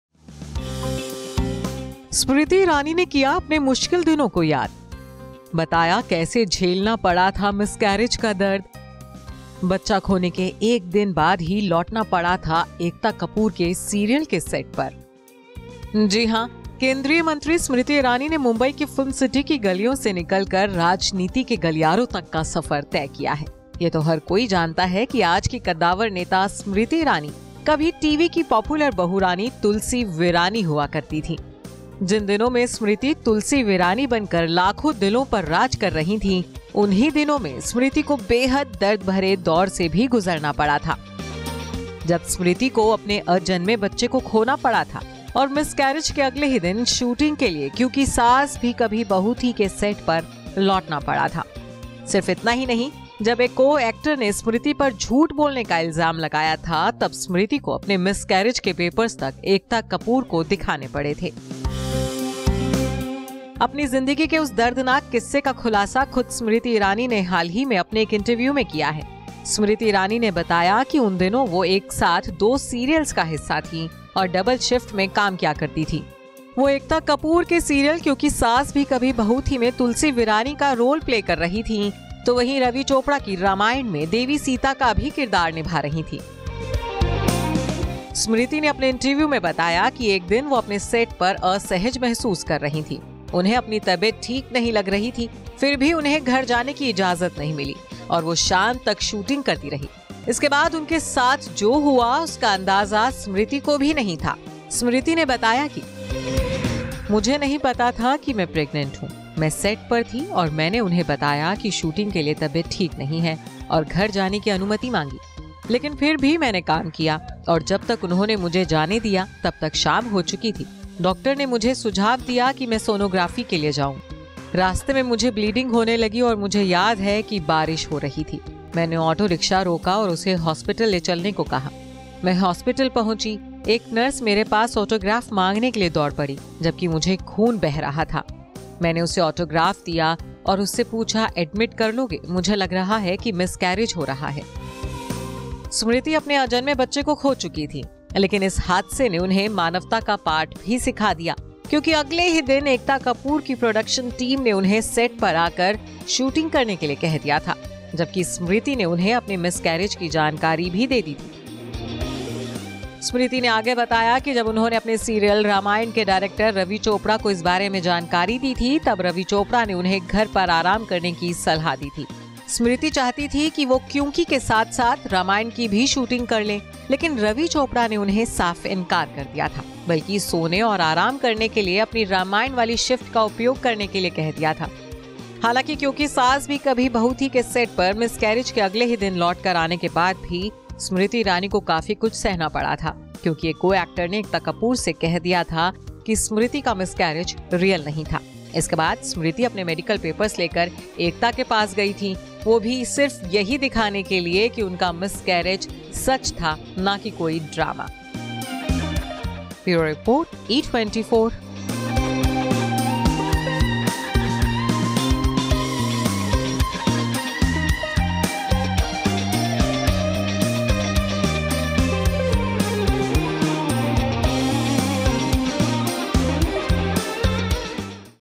जब सेट पर बिगड़ गई थी Smriti Irani की तबियत, मिसकैरिज के अगले ही दिन शूटिंग के लिये बुला लिया था।
जिन दिनों में स्मृति ईरानी तुलसी विरानी बनकर लाखों दिनों पर राज कर रही थीं, उन्हीं दिनों में स्मृति को बेहद दर्दभरे दौर से भी गुज़रना पड़ा था। जब स्मृति को अपने अजन्मे बच्चे को खोना पड़ा था और मिसकैरिज के अगले ही दिन शूटिंग के लिये क्योंकि सास भी कभी बहू थी के सेट पर लौटना पड़ा । सिर्फ इतना ही नहीं, जब एक को-एक्टर ने स्मृति पर झूठ बोलने का इल्ज़ाम लगाया था, तब स्मृति को अपने मिसकैरिज के पेपर्स तक एकता कपूर को दिखाने पड़े थे।